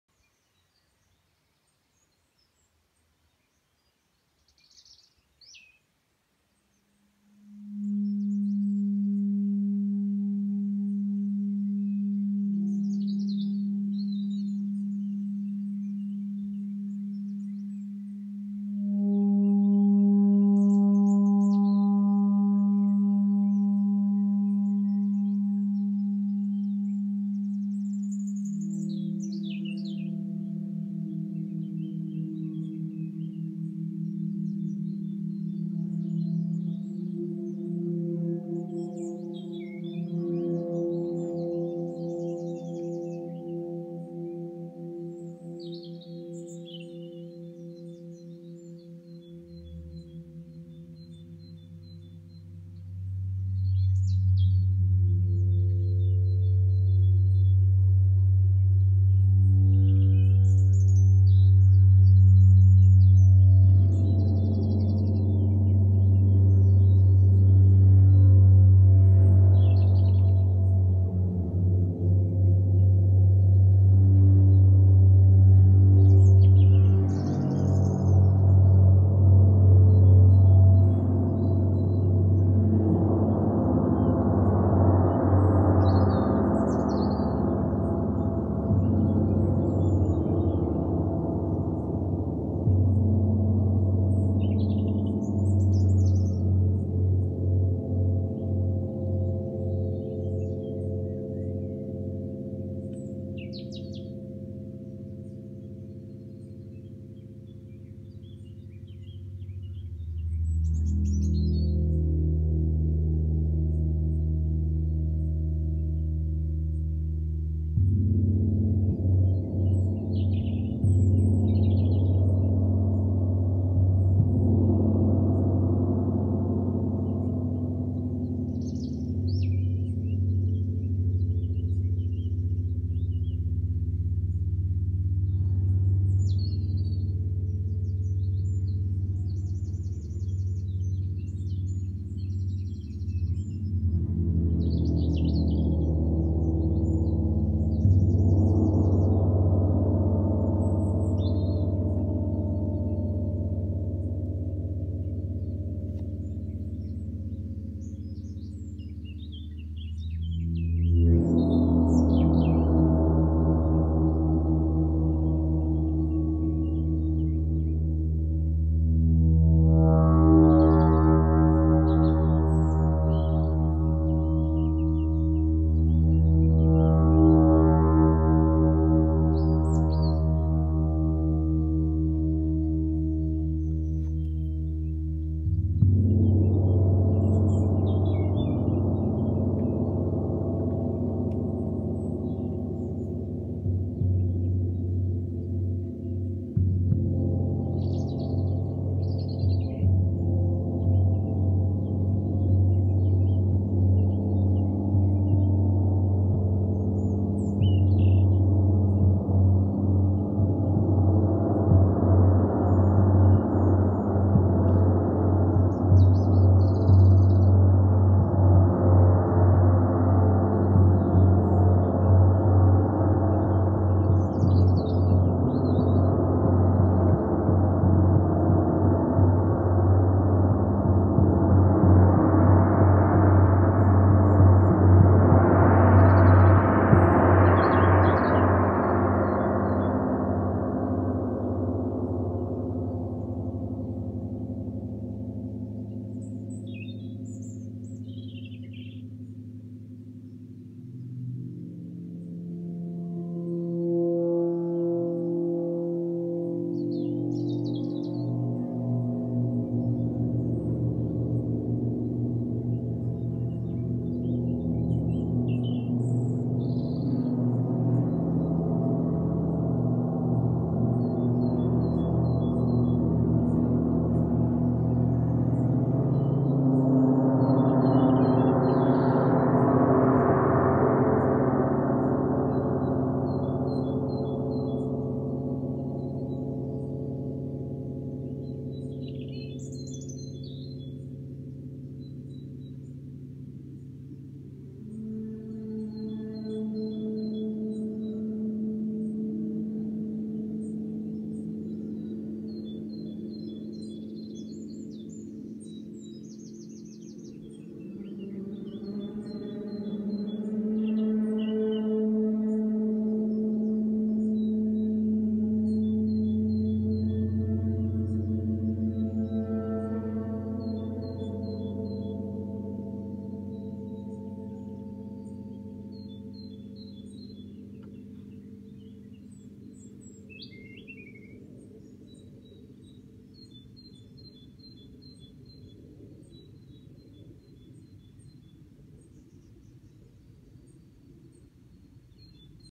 Titanium gong no 13.
Hand cold forged in 1.8mm grade 1 titanium, 47 inch diameter. The slightly thinner metal quickly produces high frequencies with smaller flumies, but will also "roar" with mallets and larger diameter flumies.  It is also light enough to be easily carried by one person.